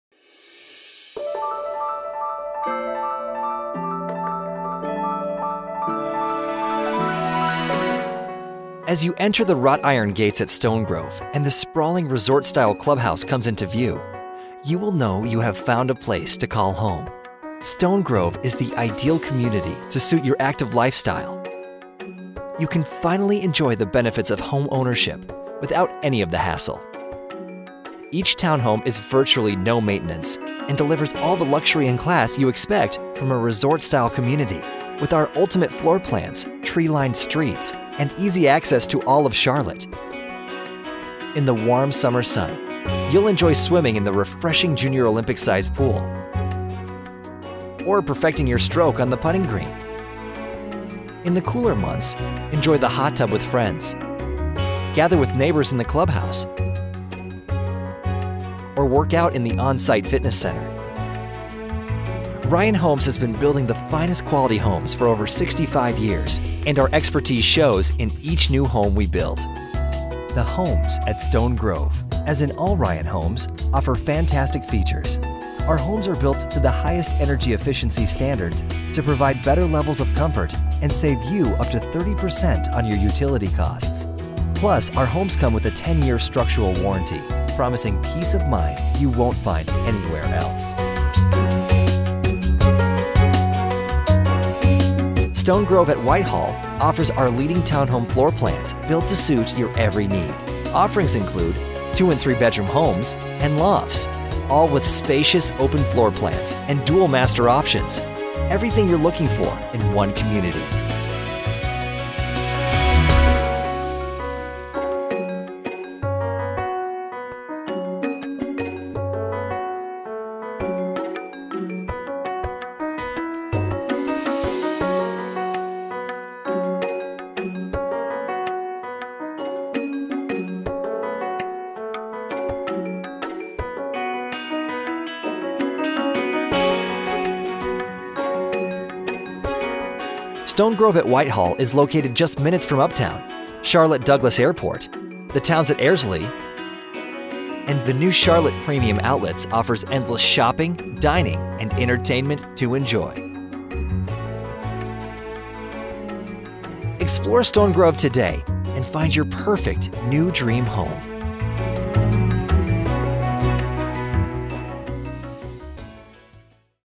New Homes at Stone Grove at Whitehall in Charlotte, North Carolina
Gated South Charlotte community, offering low maintenance townhome living & amazing amenities at an incredible value